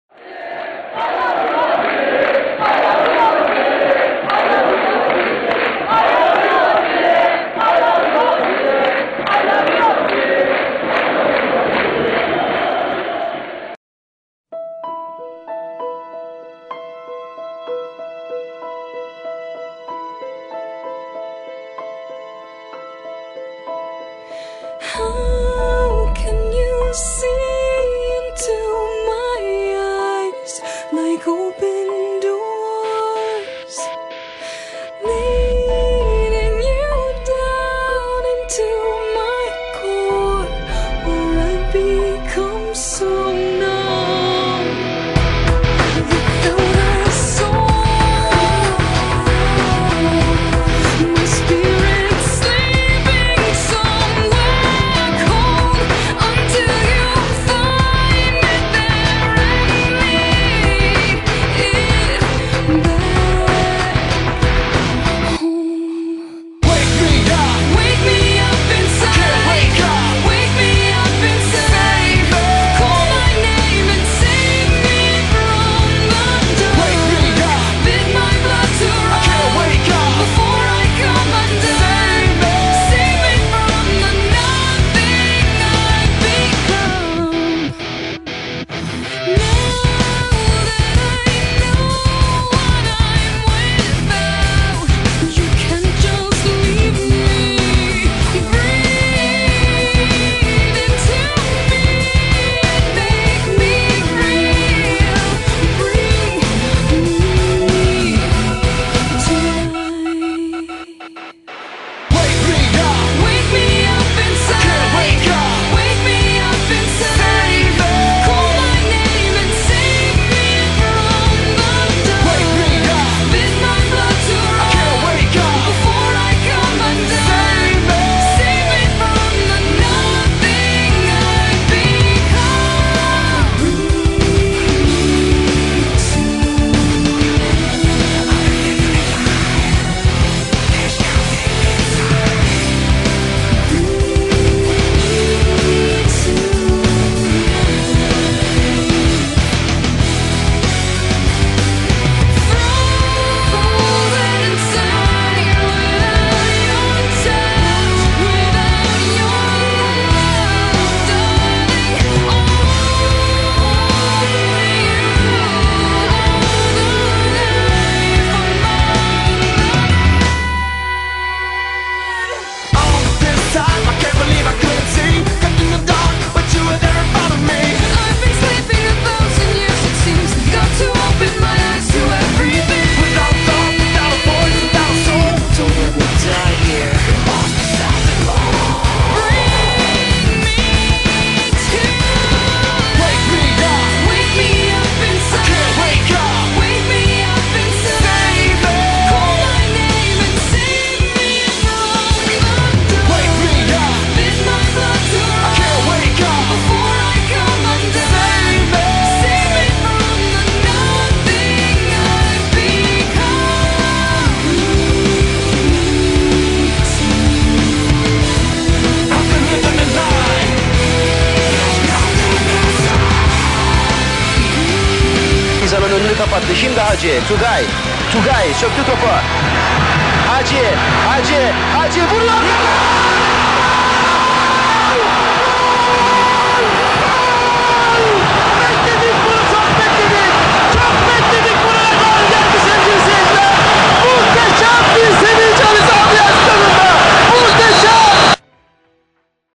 Hagi
This is a video i found on the internet.
It´s about Hagi at Galatasaray.